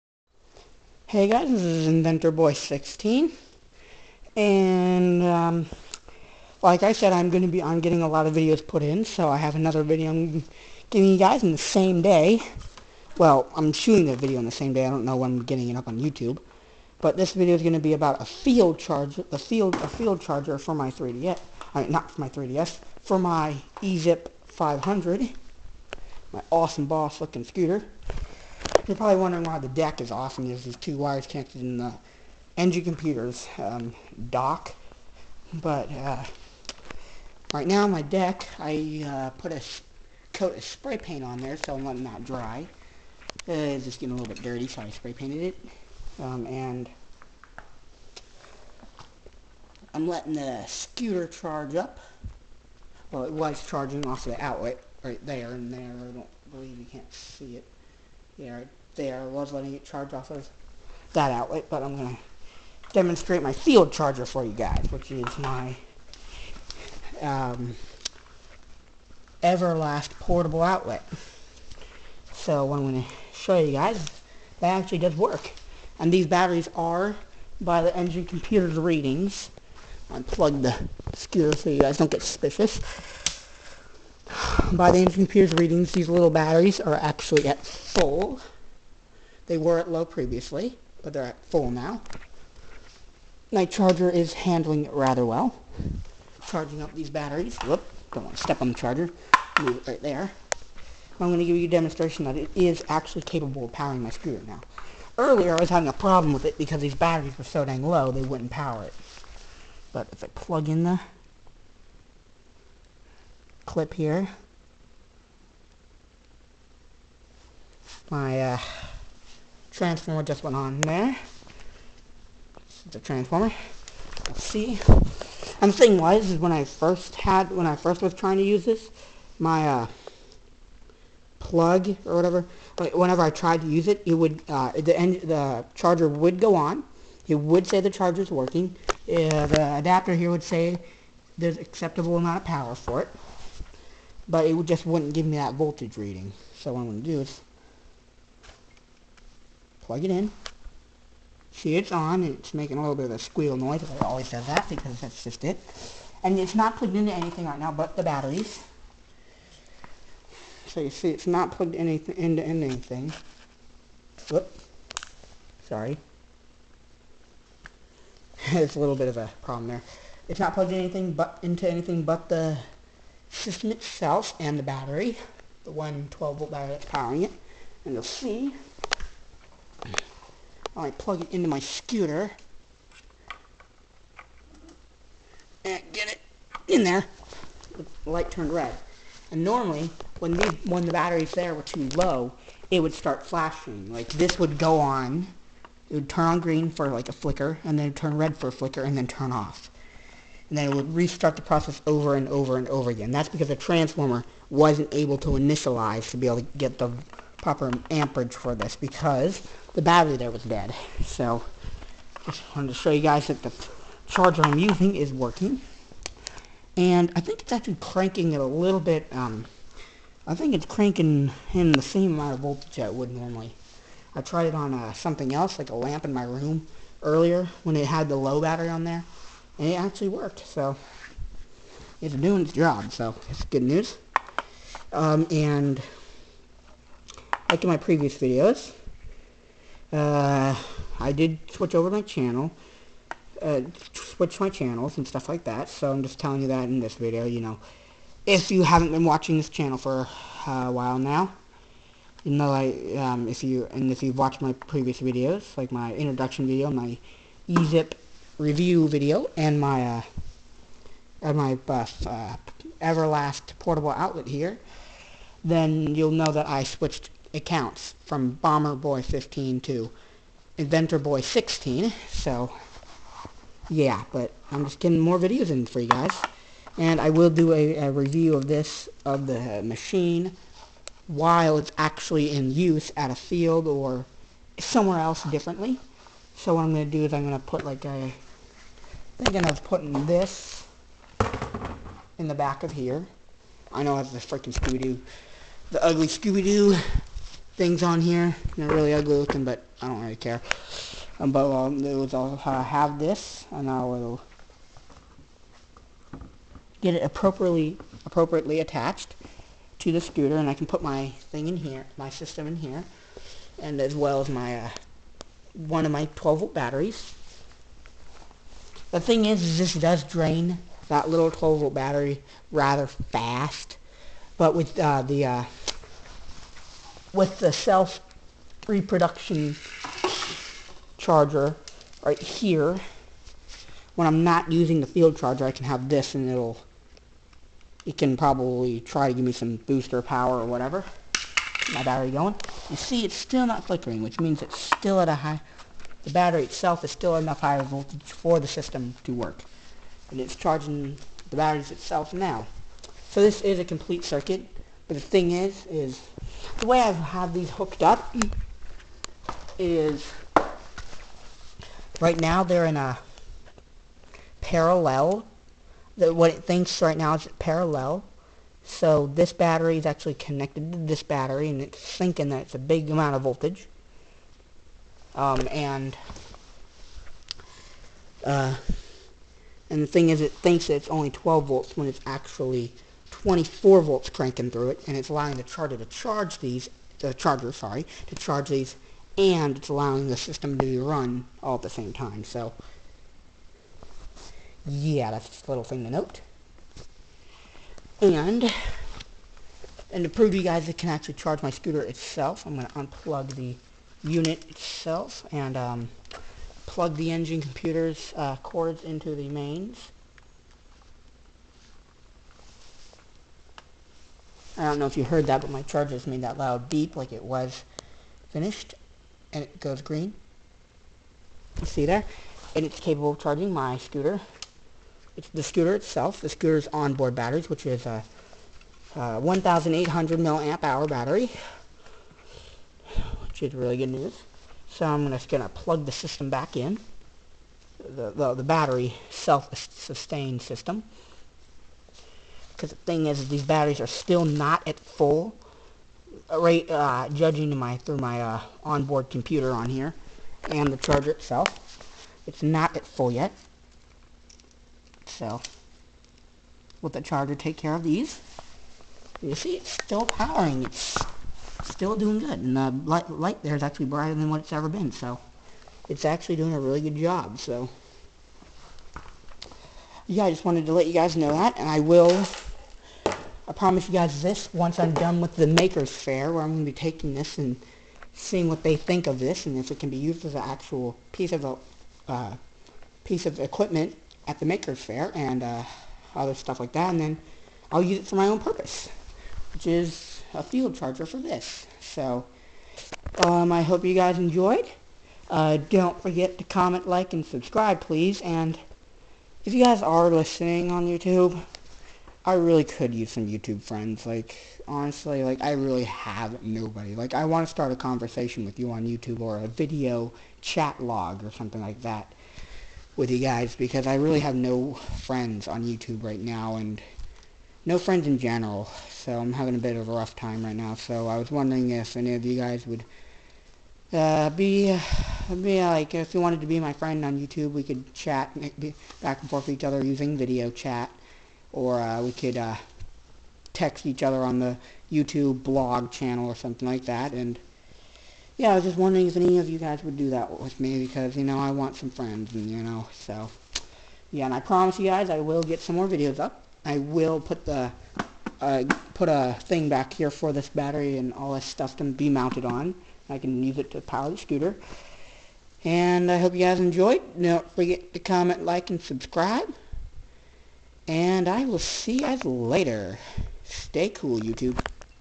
Ezip 500 field charger
This will allow me to charge my Ezip 500 on the go with no outlet need, I will give it a test after the Makers fear.